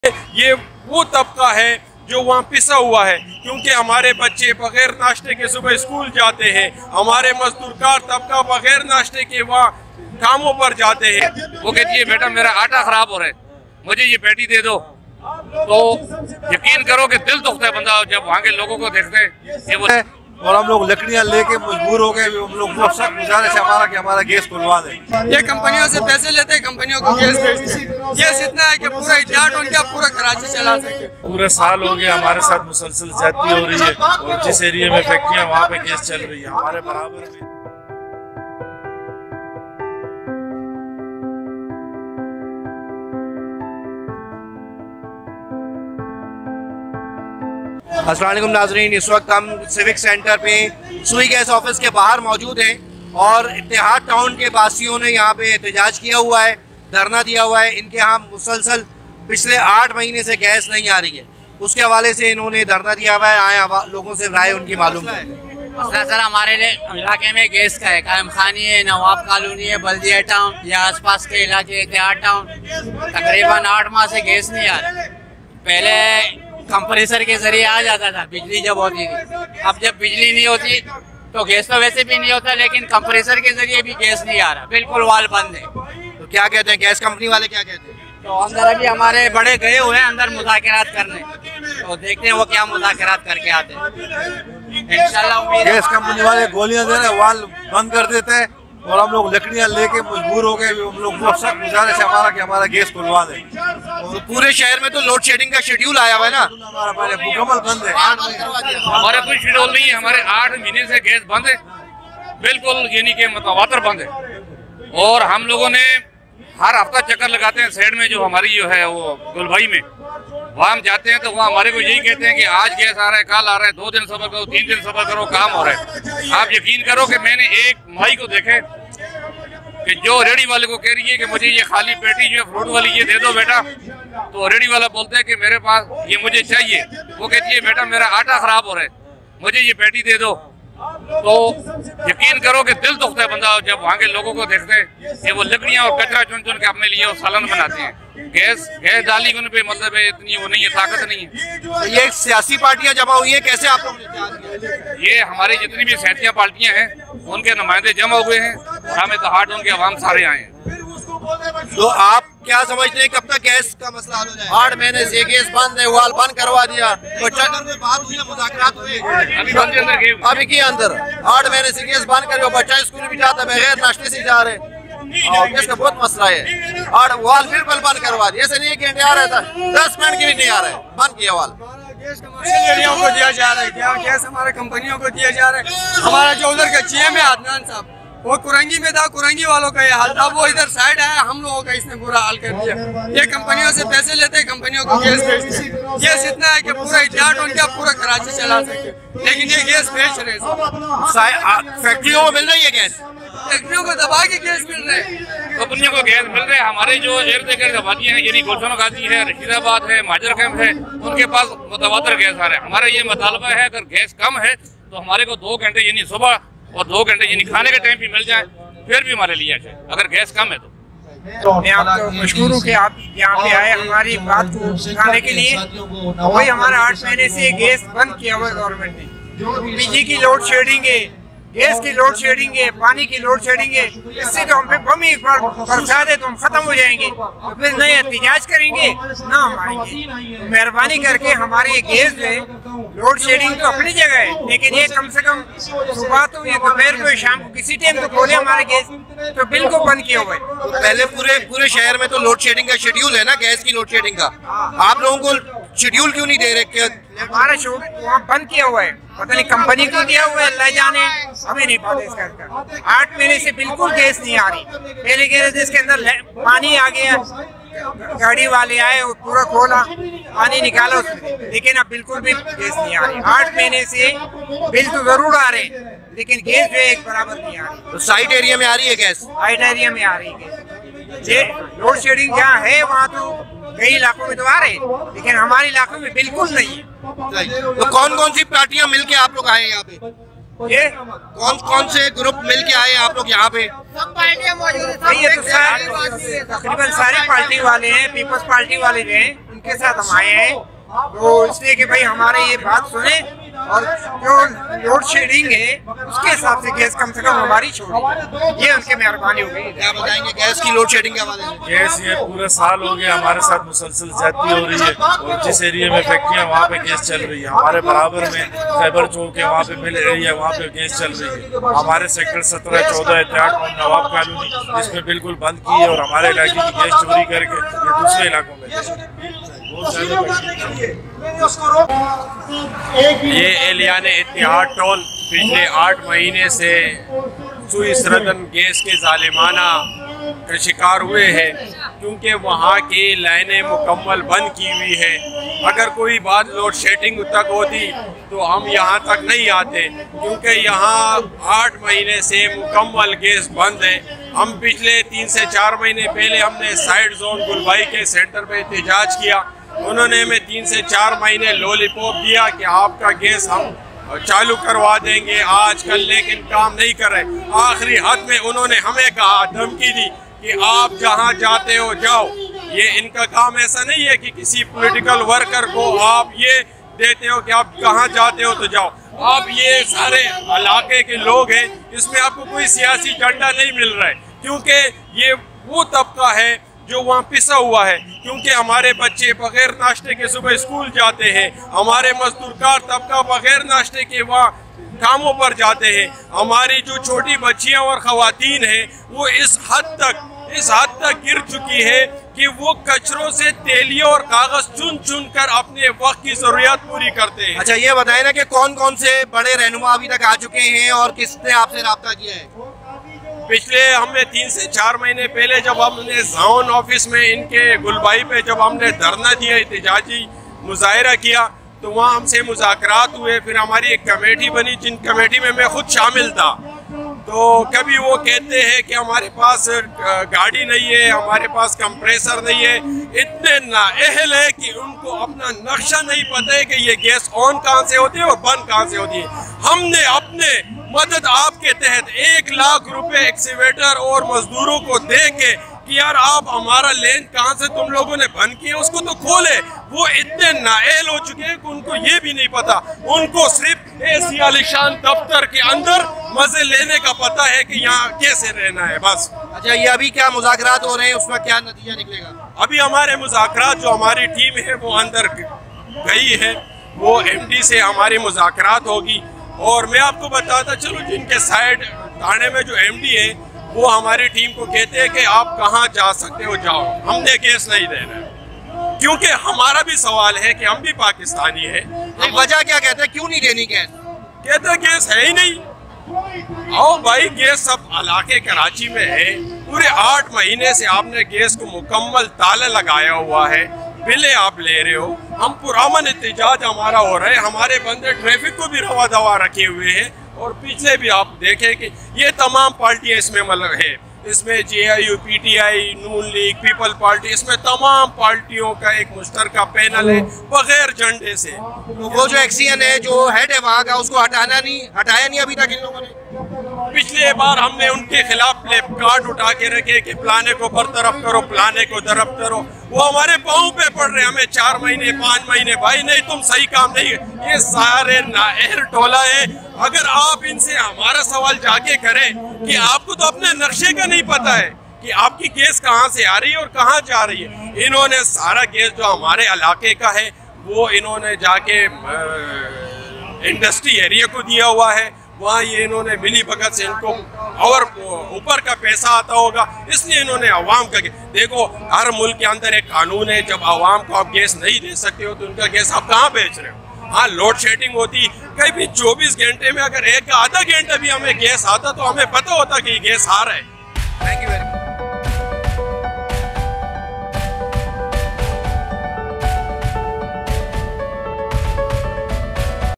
No Gas in Itehad Town, Karachi | Raabta Tv
No Gas in Itehad Town, Karachi | People protest against SSGC | Raabta Tv
اتحاد ٹاؤن کراچی میں 8 ماہ سے گیس کی فراہمی معطل